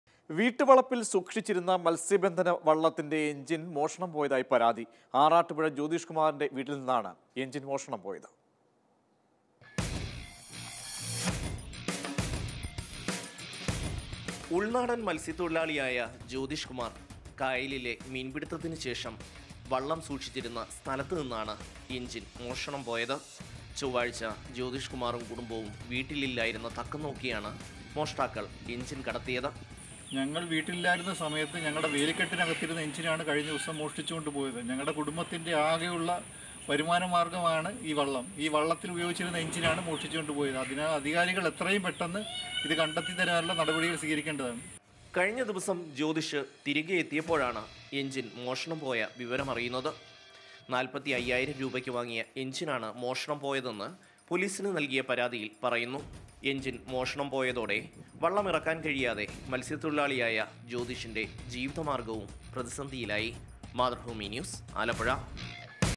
മത്സ്യബന്ധന വള്ളത്തിന്റെ എൻജിൻ മോഷണം പോയതായി പരാതി| Mathrubhumi News
വീട്ടുവളപ്പിൽ സൂക്ഷിച്ചിരുന്ന മത്സ്യബന്ധന വള്ളത്തിന്റെ എൻജിൻ മോഷണം പോയതായി പരാതി. ആറാട്ടുപുഴ ജ്യോതിഷ്കുമാറിൻറെ വീട്ടിൽ നിന്നാണ് എൻജിൻ മോഷണം പോയത്

Happy viewing!